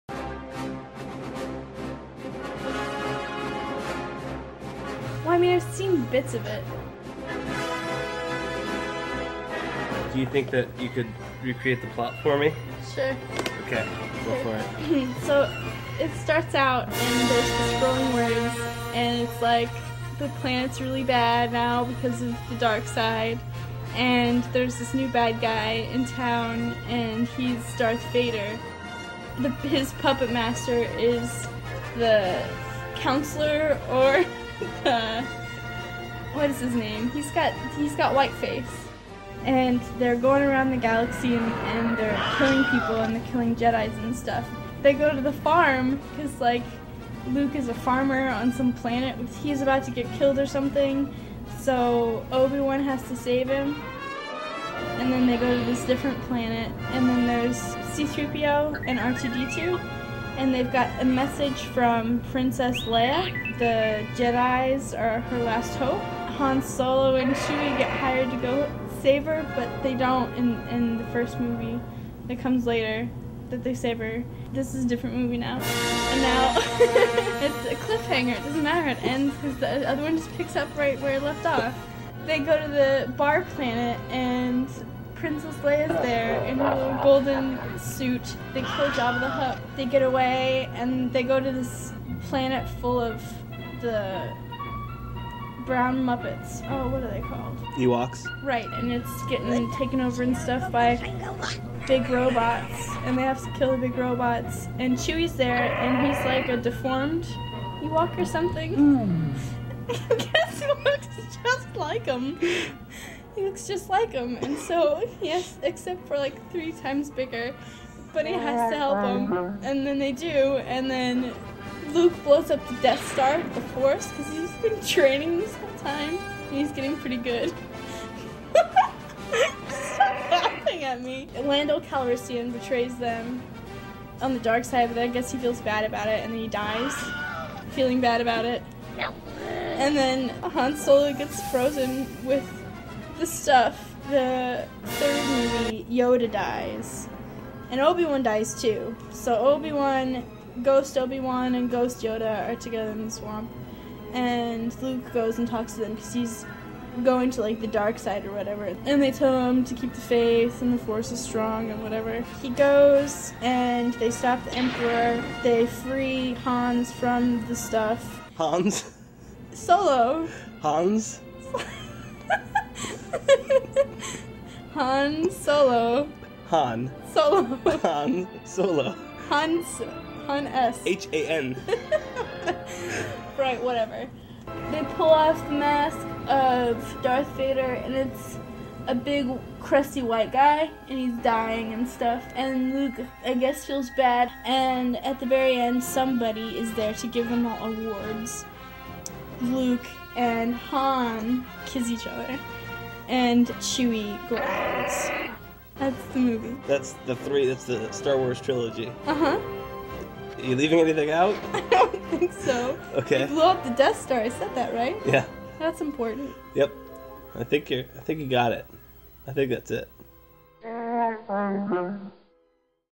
Star Wars: Retold (by someone who hasn't seen it)
Joe Nicolosi recorded his friend Amanda as she retells the story from the tiny amount shes seen with hilarious results.